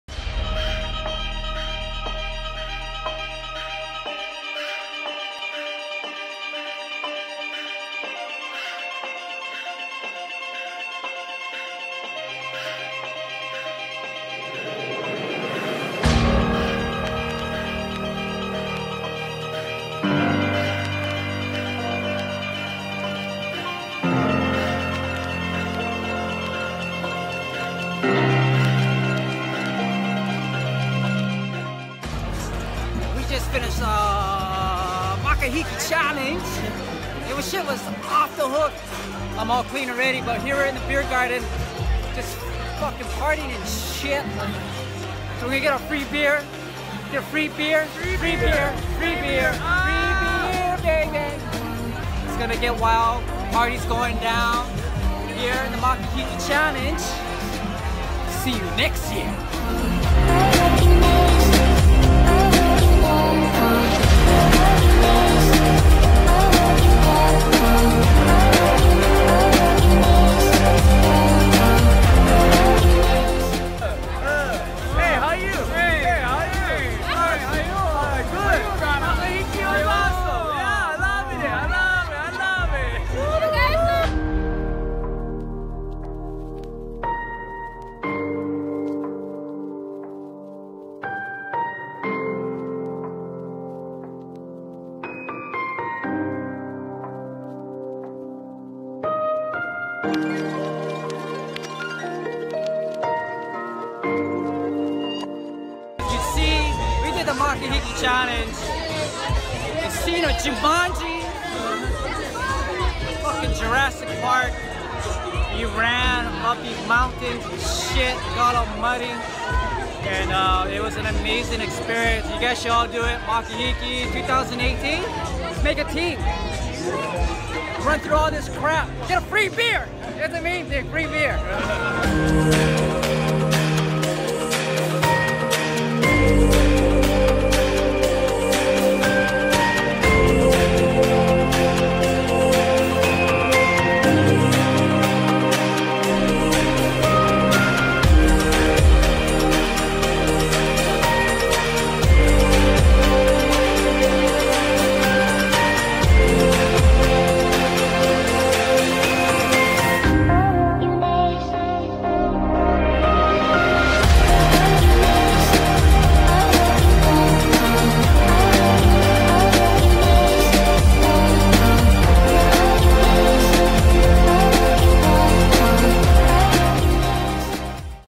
Makahiki Challenge 2017 - 5k Obstacle Mud Run
Makahiki Challenge 2017  5k Obstacle Mud Run held at Kuuloa Ranch. The scene of your favorite movies like most recently Jumanji, Jurassic Park, Jurassic World, Kong Skull Island, 50 First Dates, and more!

An awesome obstacle course through iconic Hollywood scenery and props in the valley of Kuuloa Ranch on the island of Oahu in Hawaii.

If your a fan of Spartan Race, Warrior Dash, Zombie Run, Savage Race etc than this is one you gotta try.